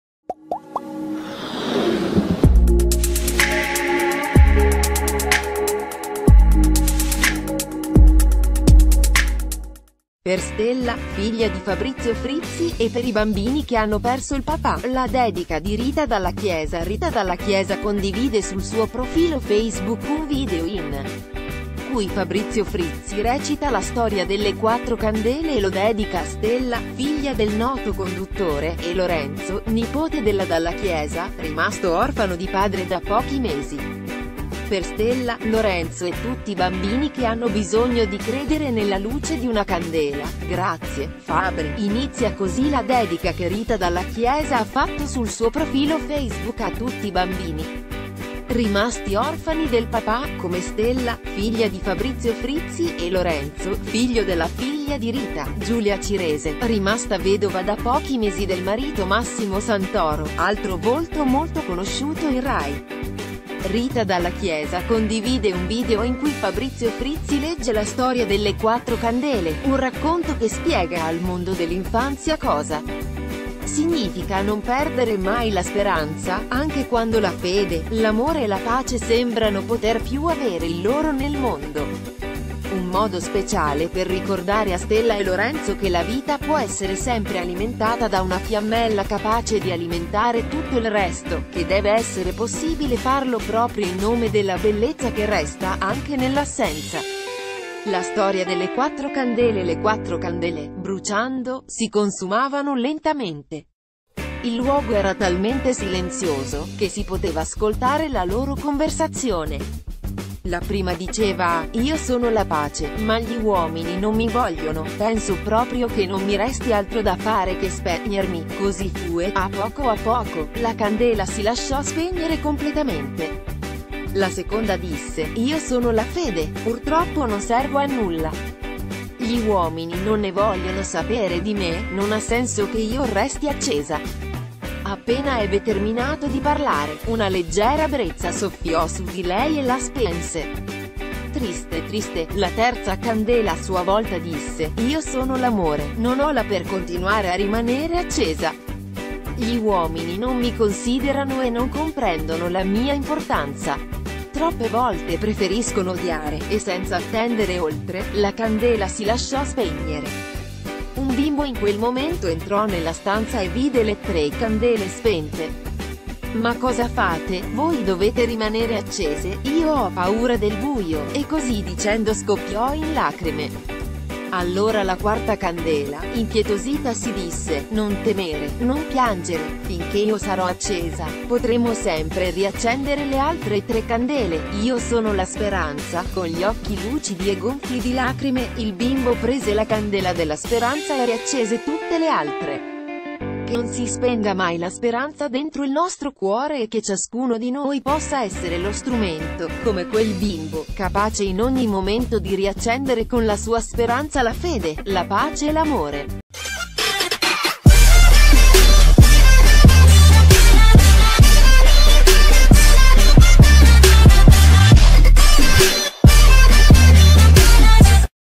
“Per Stella, figlia di Fabrizio Frizzi, e per i bambini che hanno perso il papà”, la dedica di Rita
“Per Stella, figlia di Fabrizio Frizzi, e per i bambini che hanno perso il papà”, la dedica di Rita Dalla Chiesa . “Per Stella, figlia di Fabrizio Frizzi, e per i bambini che hanno perso il papà”, la dedica di Rita Dalla Chiesa   Rita Dalla Chiesa condivide sul suo profilo Facebook un video in cui Fabrizio Frizzi ... ...